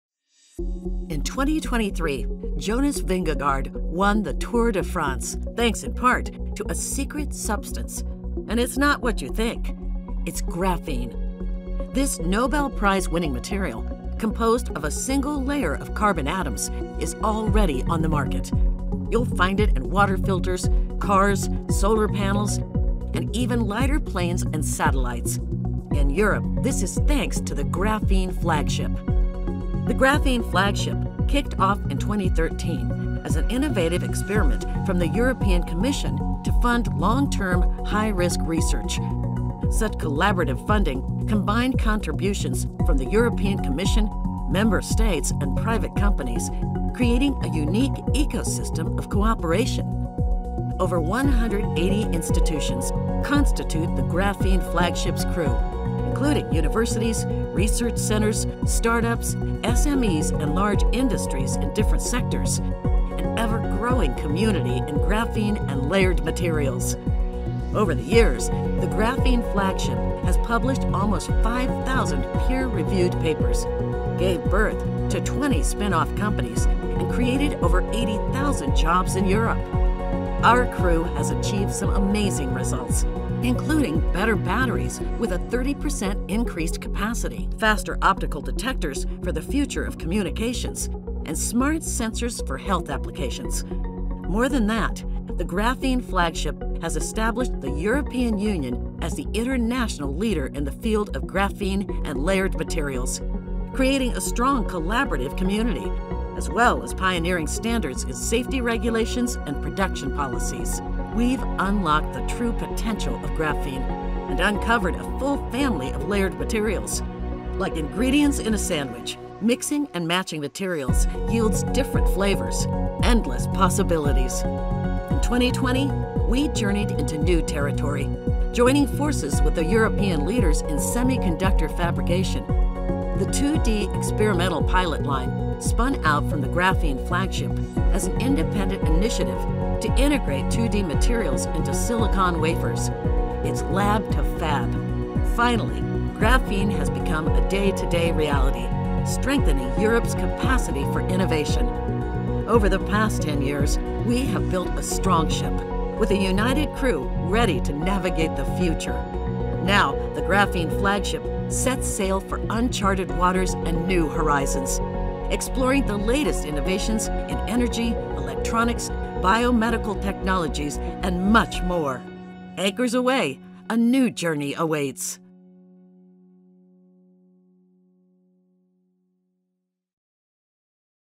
Graphene Flagship: A decade of success
The Graphene Flagship kicked off in 2013 as an innovative experiment from the European Commission to fund long-term, high-risk research. Such collaborative funding combined contributions from the European Commission, Member States and private companies, creating a unique ecosystem of cooperation.

Over the years, the Graphene Flagship has published almost 5000 peer-reviewed papers, gave birth to 20 spin-off companies and created over 80.000 jobs in Europe.

Our crew has achieved some amazing results, including better batteries with a 30% increased capacity, faster optical detectors for the future of communications and smart sensors for health applications.

More than that, the Graphene Flagship has established the European Union as the international leader in the field of graphene and layered materials, creating a strong collaborative community, as well as pioneering standards in safety regulations and production policies.

Over the past ten years we have built a strong ship, with a united crew ready to navigate the future. Now, the Graphene Flagship sets sail for unchartered waters and new horizons, exploring the latest innovations in energy, electronics, biomedical technologies and much more!

Anchors away, a new journey awaits.

► If you want to learn more about the Graphene Flagship check out